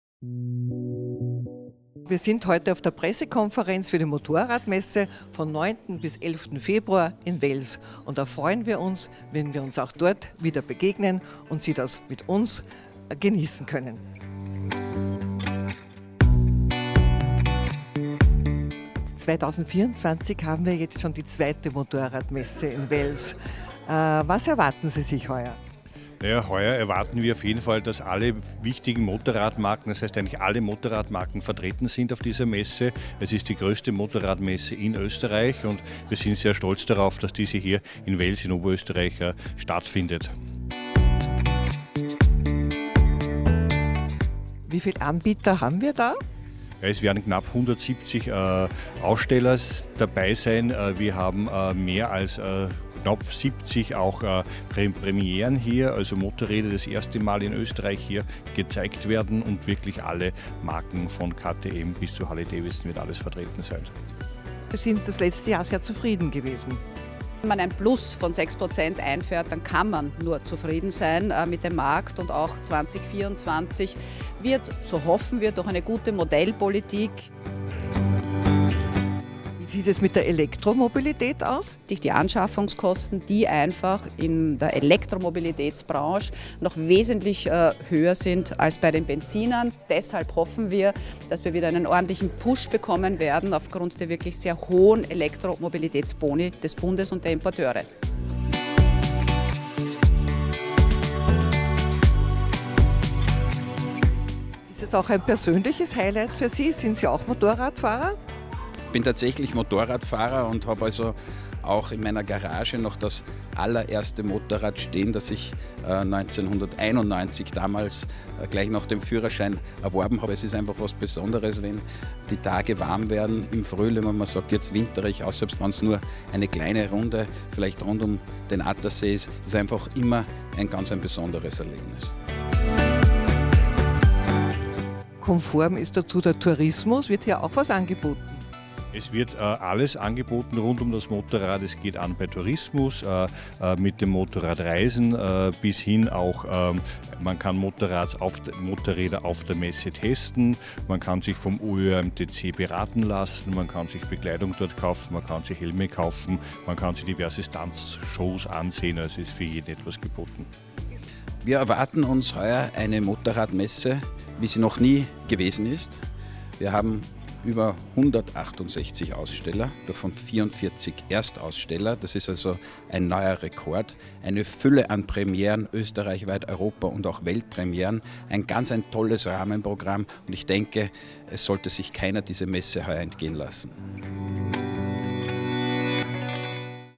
Wels-Report | MOTO AUSTRIA 2024 // Pressekonferenz, Interviews
Rund zwei Wochen vor Eröffnung der moto-austria präsentierten die Arge2Rad und die Messe Wels im OÖ Presseclub Linz das Konzept der moto-austria 2024. Österreichs Motorrad- und Rollermesse, die von 9. – 11. Februar 2024 in der Messe Wels über die Bühne geht.
Wir sprachen dazu mit dem Geschäftsführer der Messe Wels Direktor Robert Schneider, dem Messepräsidenten Dr. Peter Franzmayr und mit Mag. Karin Munk von der arge2Rad...